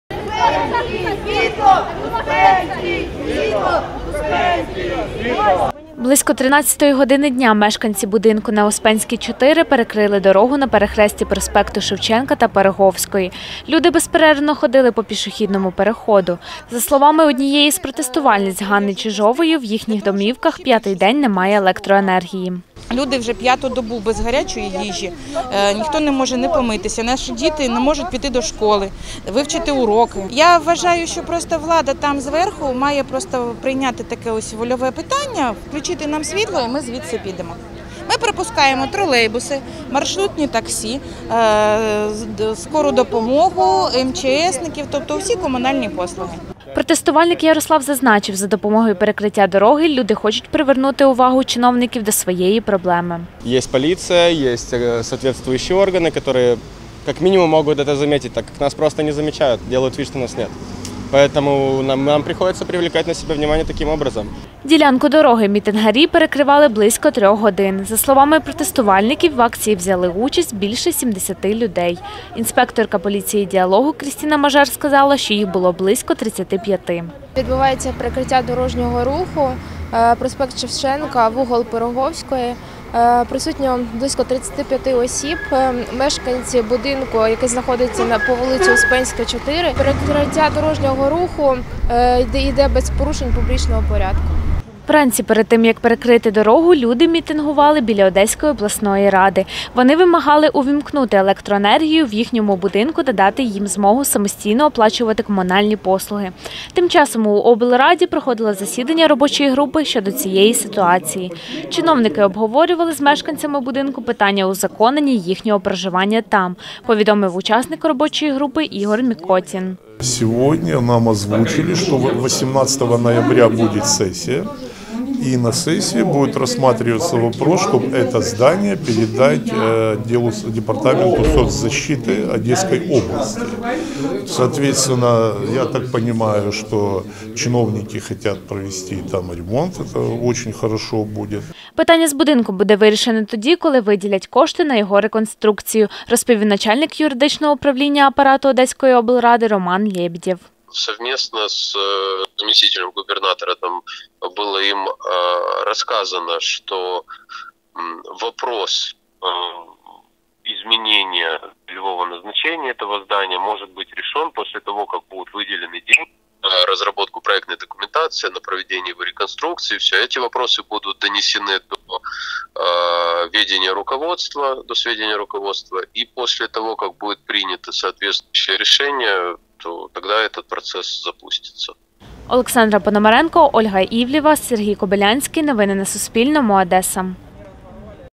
В Одесі мітингували переселенці та ветерани АТО. Що відбувається
Переселенці та ветерани АТО близько трьох годин перекривали дорогу на перехресті проспекту Шевченка та вулиці Пироговської. Протестувальники, що мешкають у будинку на Успенській,4, повідомили, що у їхніх квартирах п’ятий день немає електроенергії. Представники облради пообіцяли людям розглянути проблему на сесії облради 18 листопада. Детальніше – в сюжеті.